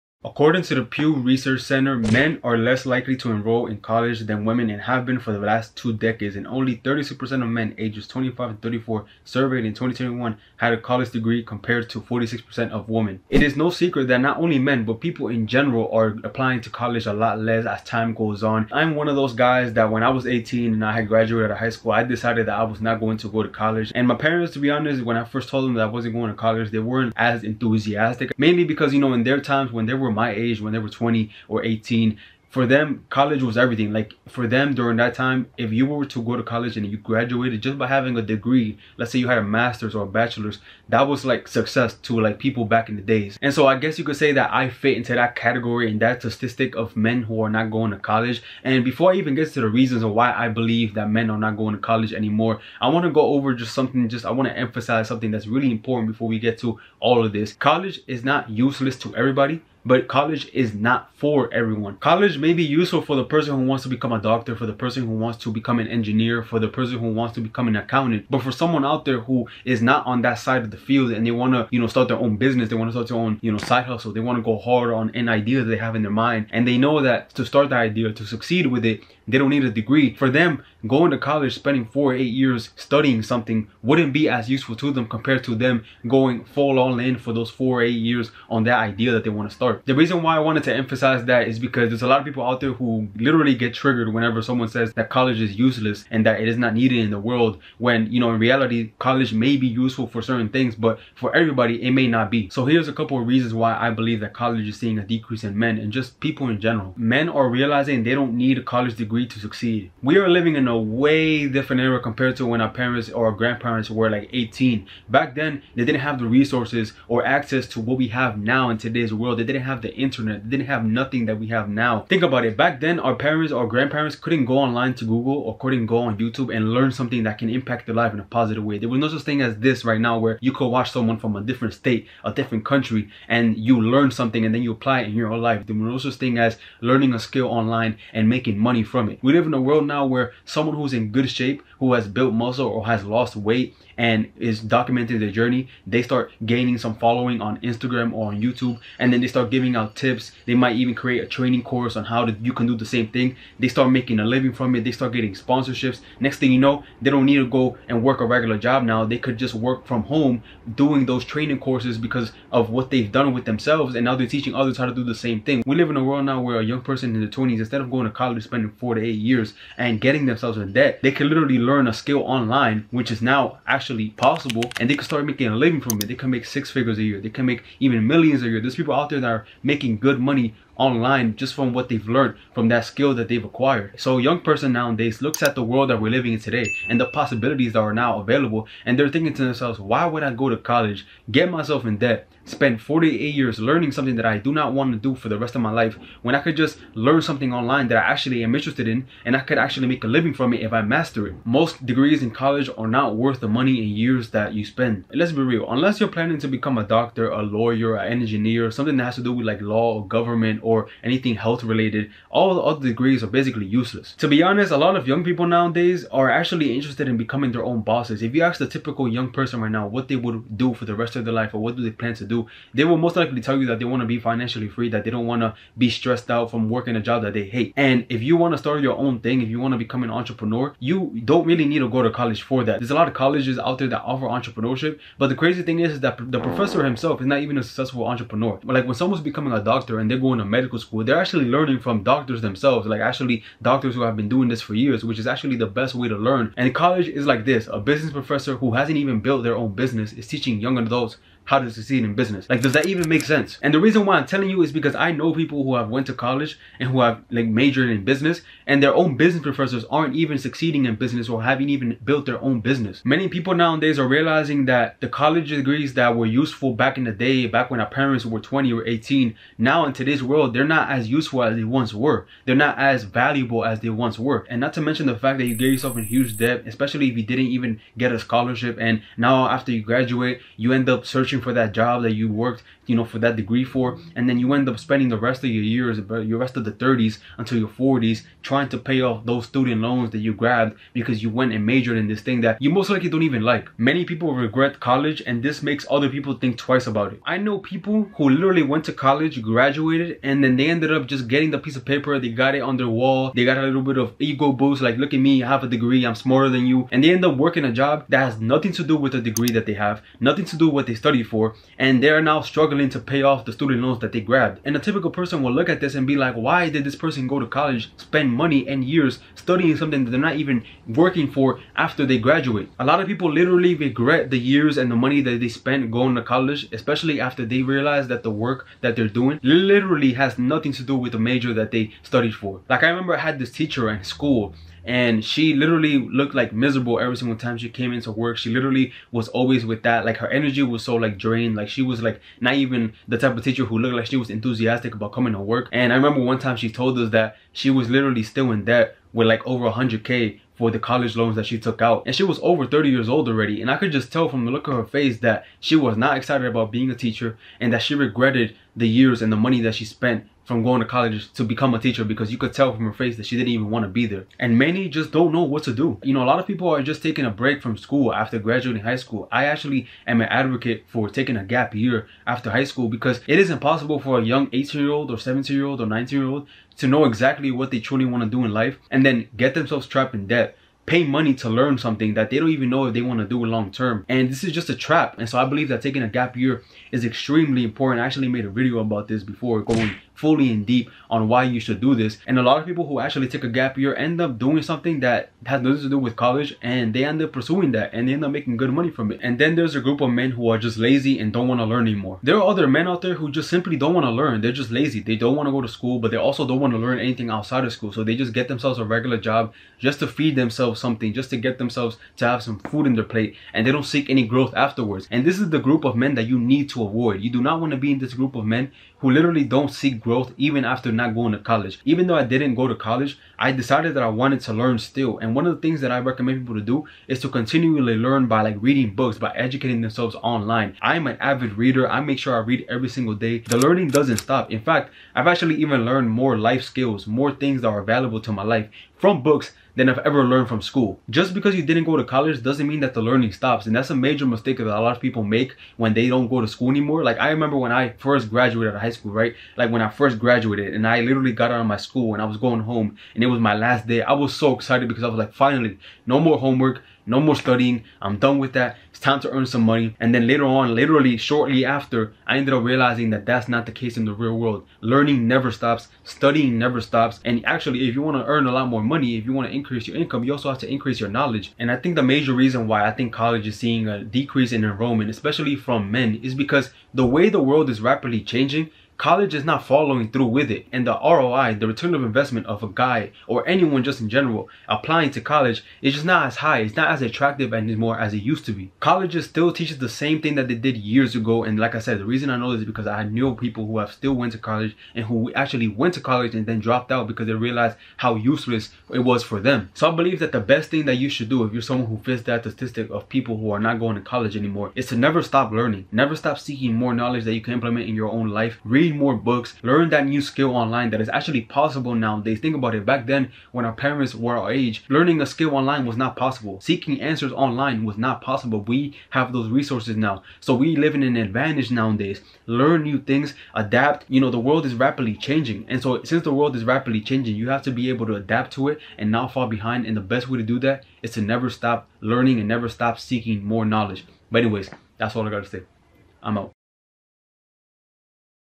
Modern Men Are Abandoning College
It is no secret that colleges are seeing a decline in people, specifically men, enrolling. The world is rapidly changing and colleges doesn't seem to following. College doesn't seem to be as attractive as it once was many years ago. Here is why modern men are abandoning college.